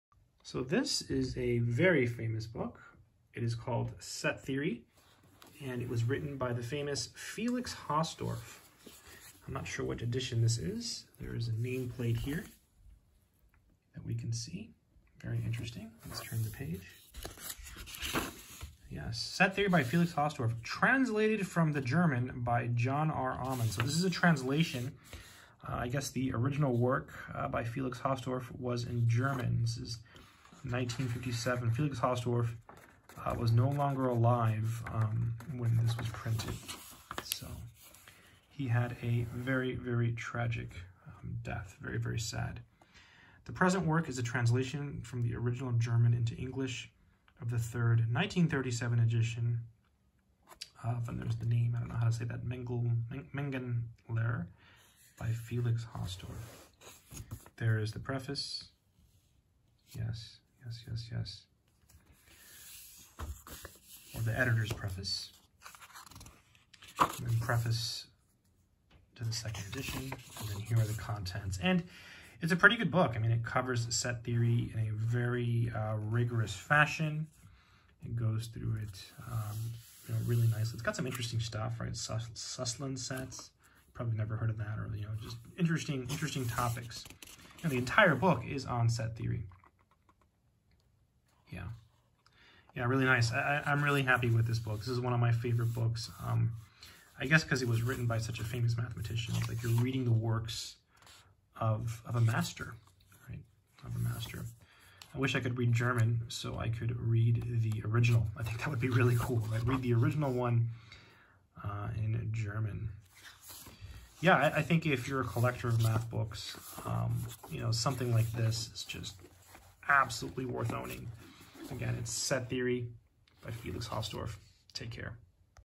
Set Theory by Hausdorff
In this video I talk about a legendary book. This is Set Theory by Felix Hausdorff. I hope this video is helpful.

Here it is on amazon